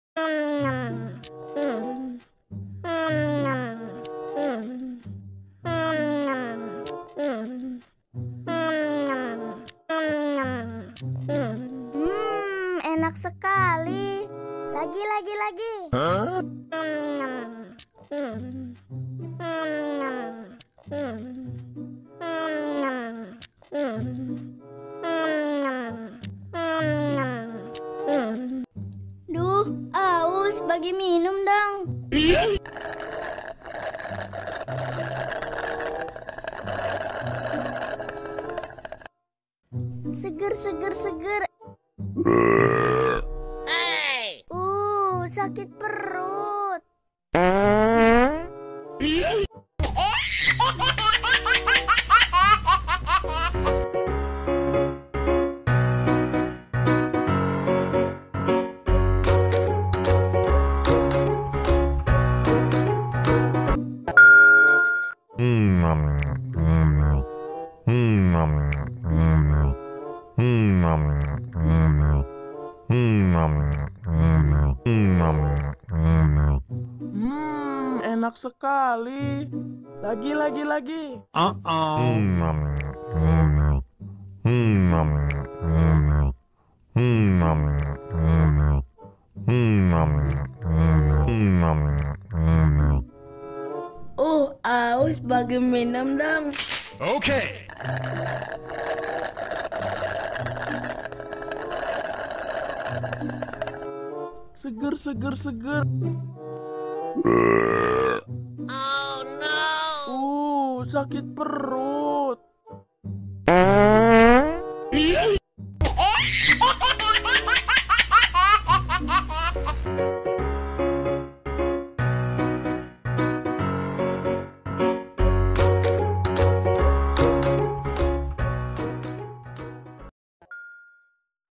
Ondel-ondel Makan Lolipop, ondel-ondel lucu, ondel-ondel betawi.
Halo teman-teman!
seperti biasa hiburan kali ini tentang ondel-ondel lagi yah yg pastinya seru dan lucu.